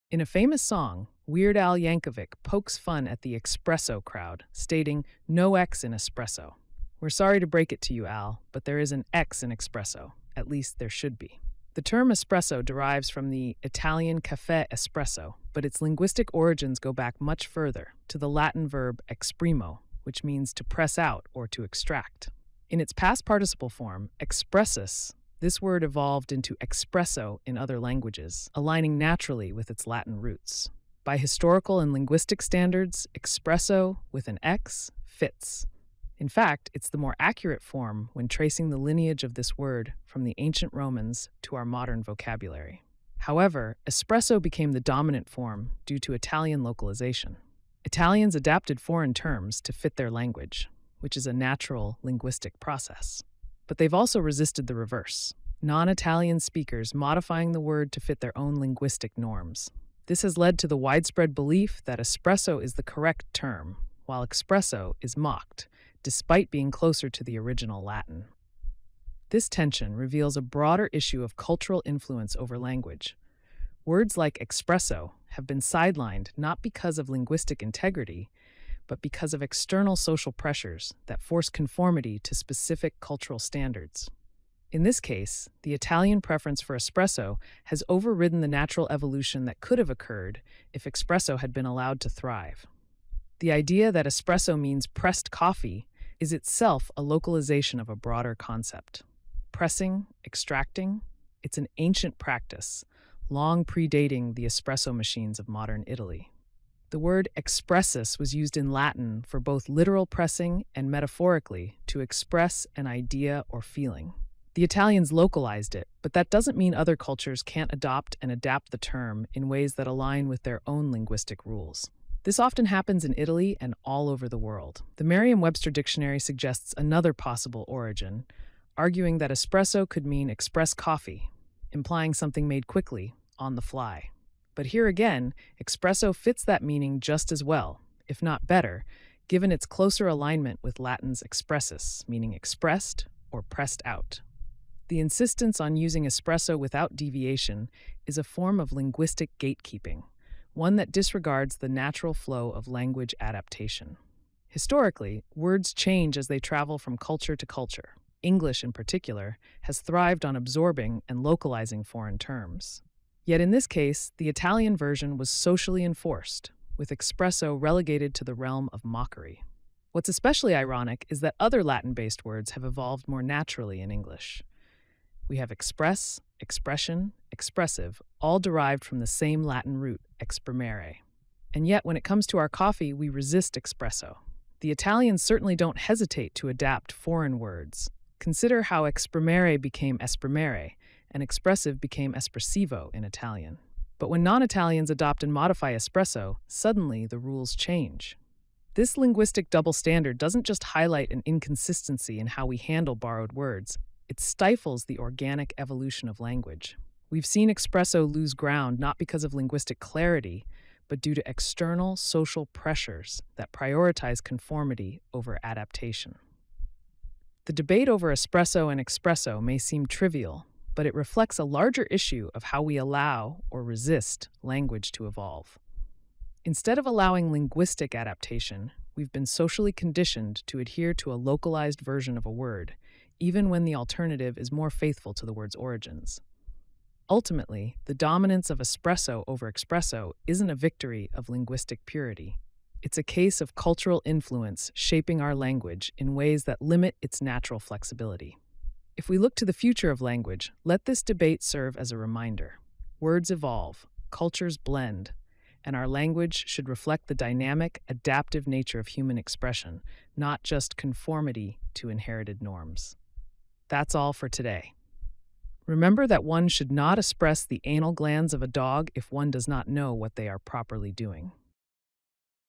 What is the Correct Pronunciation and Spelling: Espresso or Expresso?
In this video, we dive deep into the debate over "espresso" versus "expresso" and challenge the popular misconception that the latter is incorrect. While Weird Al Yankovic famously poked fun at the "expresso" crowd, we reveal that linguistically and historically, there should be an X in expresso! The word's roots trace back to the Latin "expressus," meaning "pressed out" or "extracted," and expresso actually aligns more closely with this origin.

We explore how the dominance of "espresso" is less about linguistic purity and more about Italian cultural influence shaping the term, leading to the marginalization of expresso. Despite this, language is fluid, and cultures often adopt and adapt terms in their own ways. So why not give expresso its due?

Join us as we unravel the fascinating linguistic journey of this beloved coffee term, challenge linguistic gatekeeping, and remind you not to espress your dog's anal glands unless you know what you're doing!